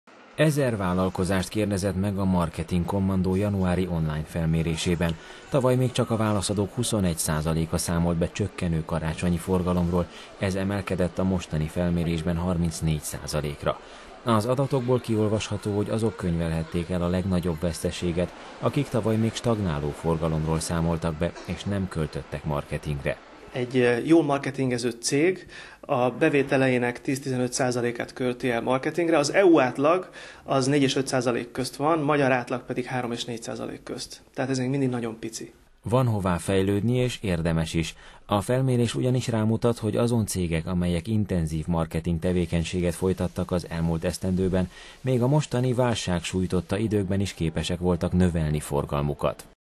KKV marketing kutatás beszámolója a Duna TV-ben (2010.01)
Karácsonyi forgalmi trendek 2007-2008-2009 a kkv-k körében. Több infó